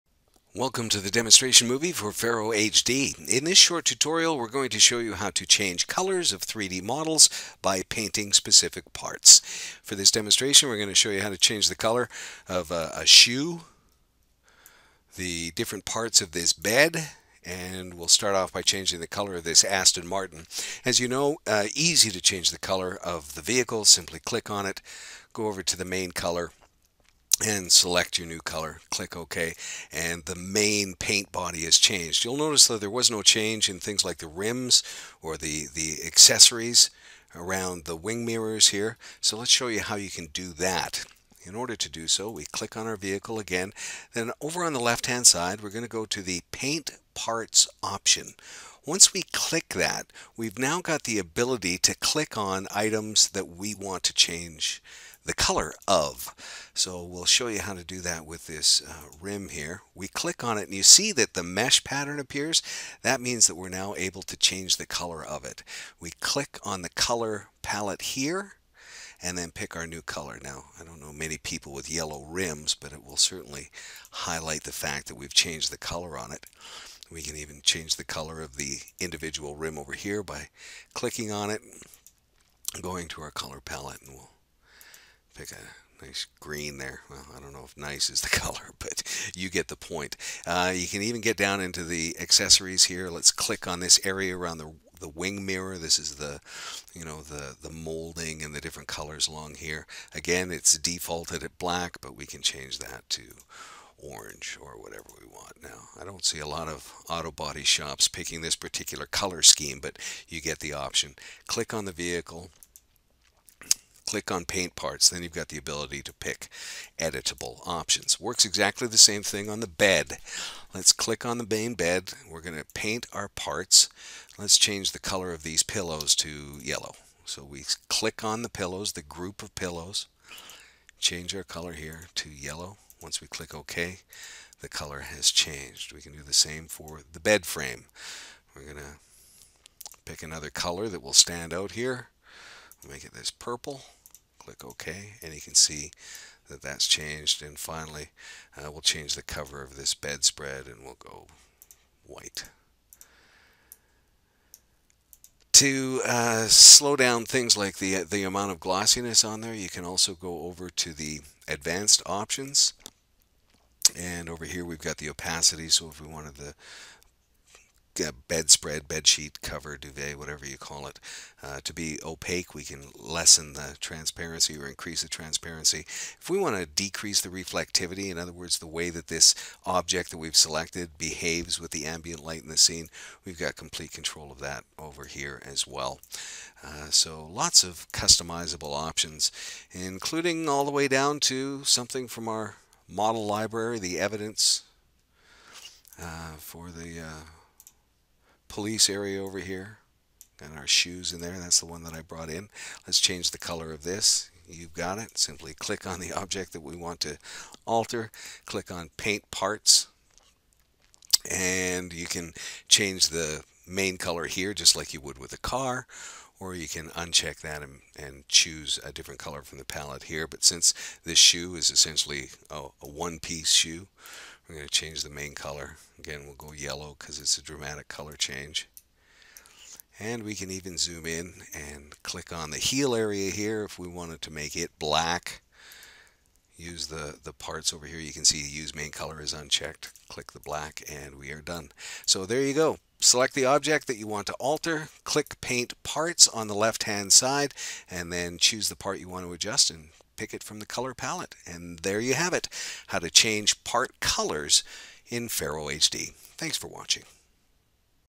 FARO HD : How to Change Color of 3D Models
In this tutorial we are going to demonstrate how to change colors of 3D models by painting specific parts of our models.

Software : FARO HD
Company: FARO Technologies Inc.